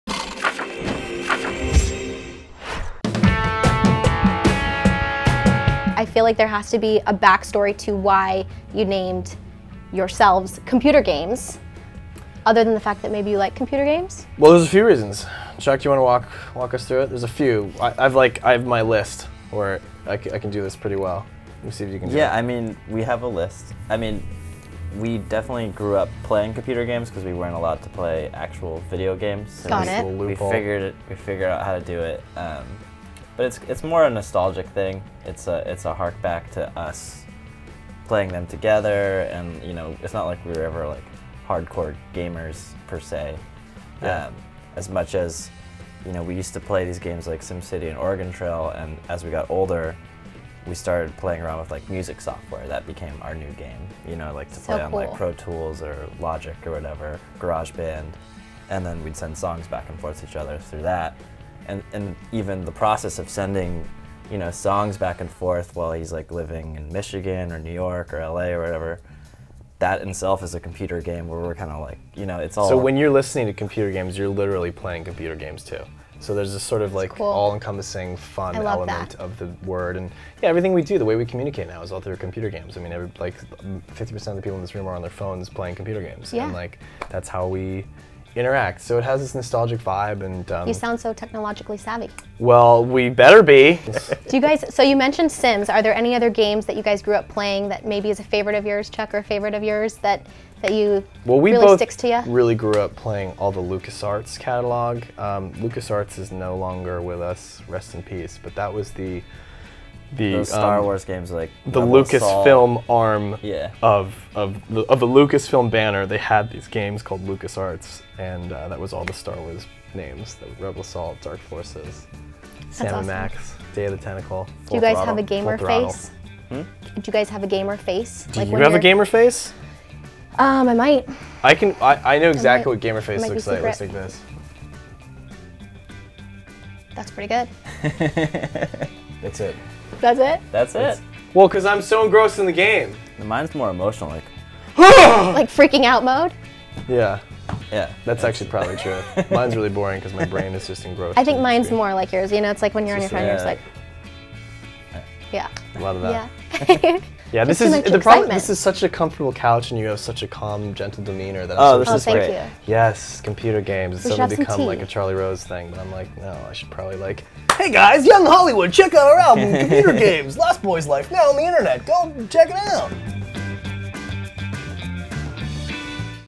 Darren Criss Shows Off His Gamer Face!
We're hanging out with Darren Criss and his brother Chuck in the YH Studio discussing their musical collaboration, Computer Games, and how it (and the name) was inspired by the technology they had growing up! They reveal their favorites games to play as kids and then show us their "gamer faces"!

Apps

2&keywords=young+hollywood

Social

Channels